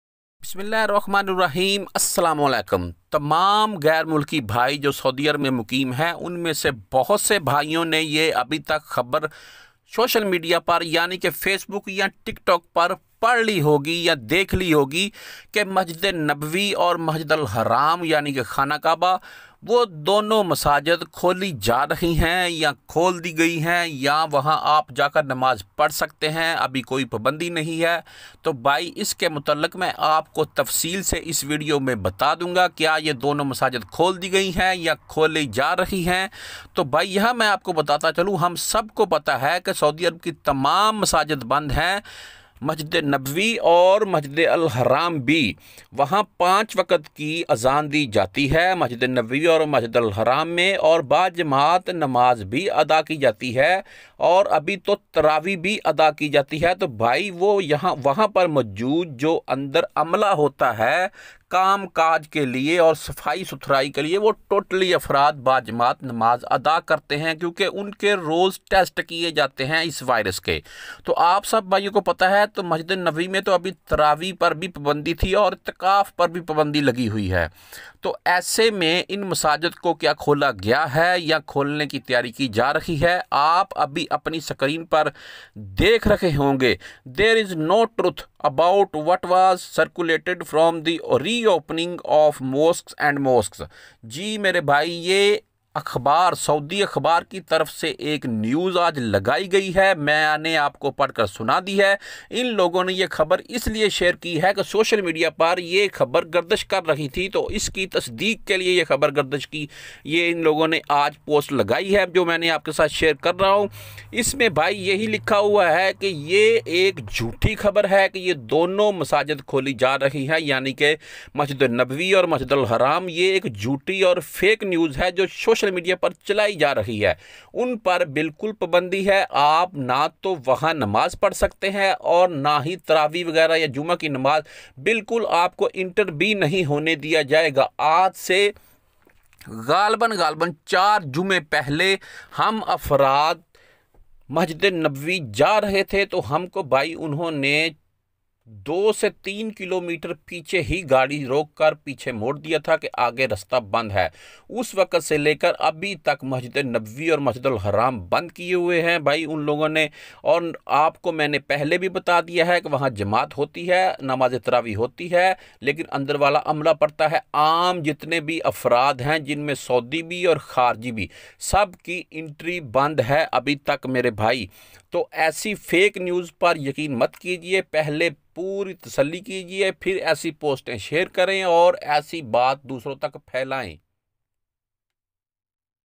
has masjid nabavi and masjid al haram been opened?, kia masjade nabvi aur khana khaba khool deye gai
has masjid nabavi and masjid al haram been opened?, kia masjade nabvi aur khana khaba khool deye gai hai , kia saudi arab ki tamam masajad khool di gai hai,